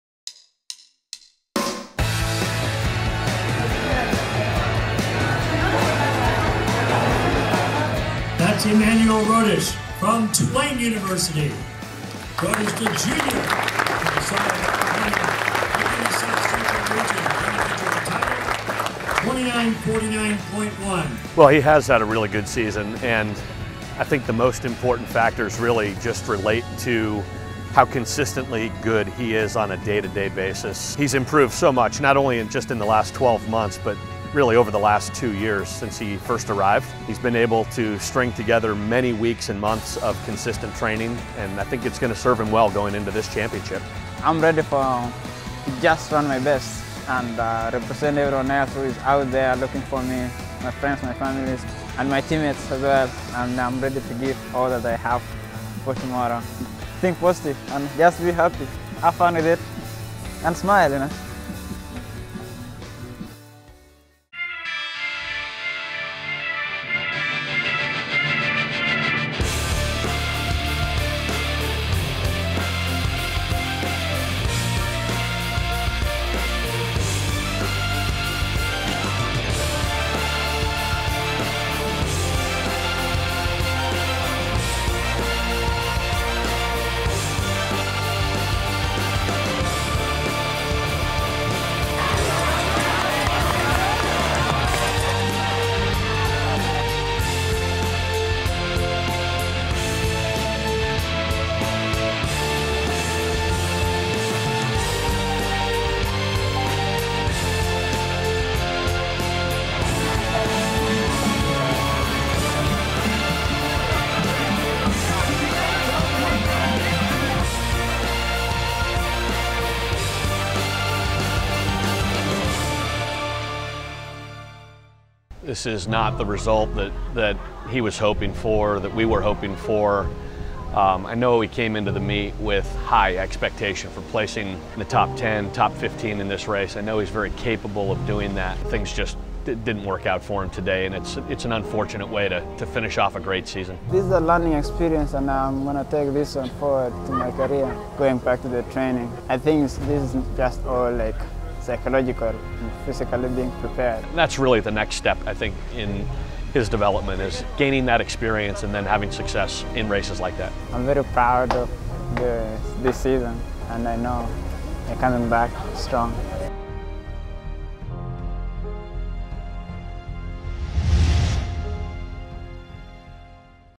Emmanuel Rotich NCAA Championships Feature
What a season! Emmanuel Rotich concluded his historic year this past weekend at the NCAA XC National Championships!
Relive Emmanuel's journey to Louisville!